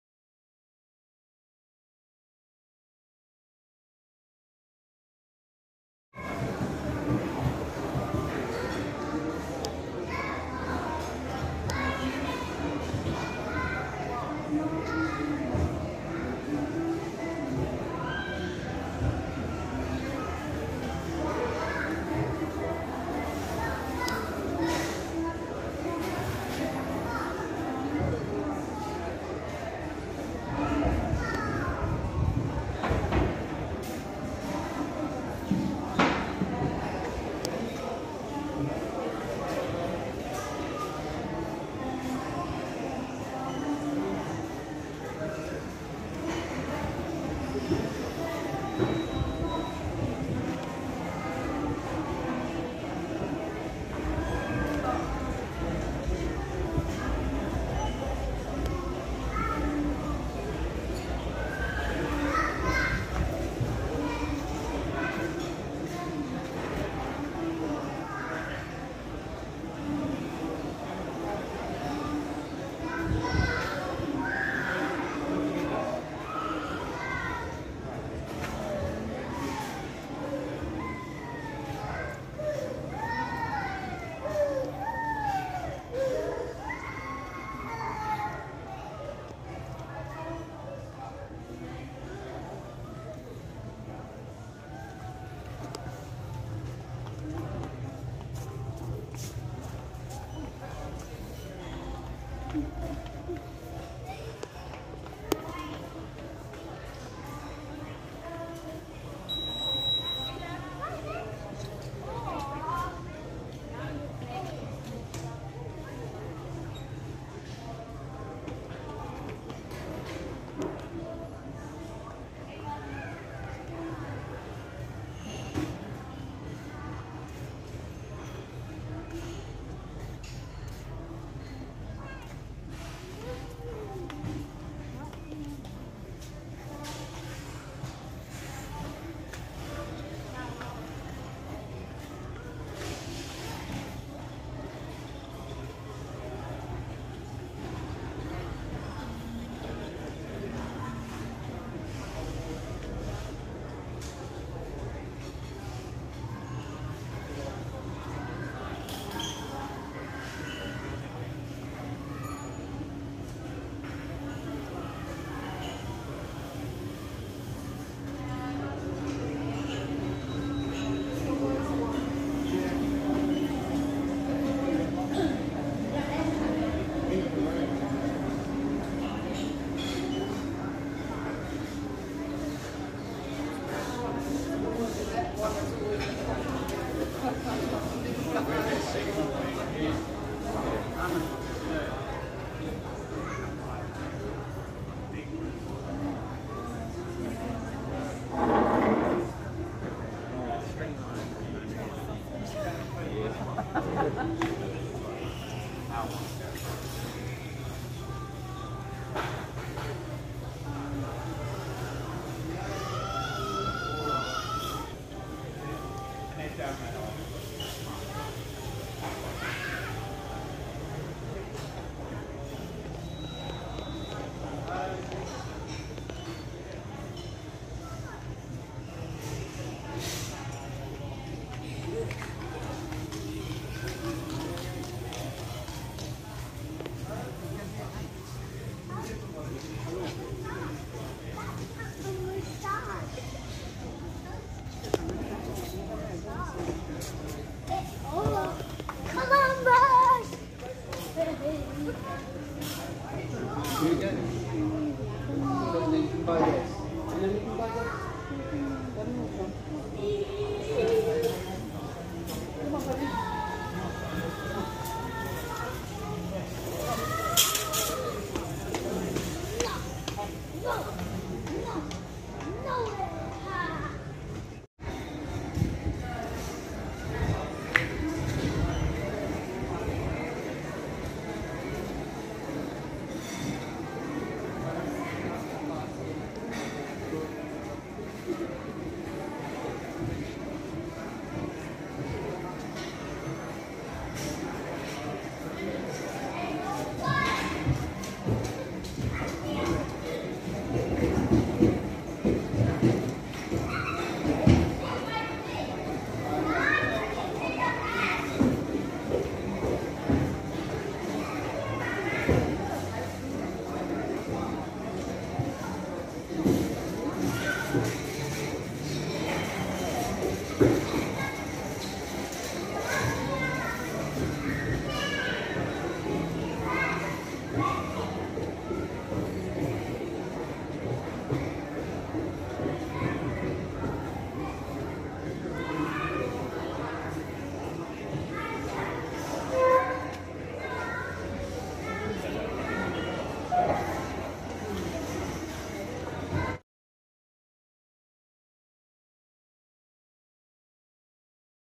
Beautiful Plants and Flowers of New Zealand
Beautiful plants and flowers in New Zealand. They are video taken in a big garden centre inside the biggest Mitre 10 Mega in Auckland New Zealand.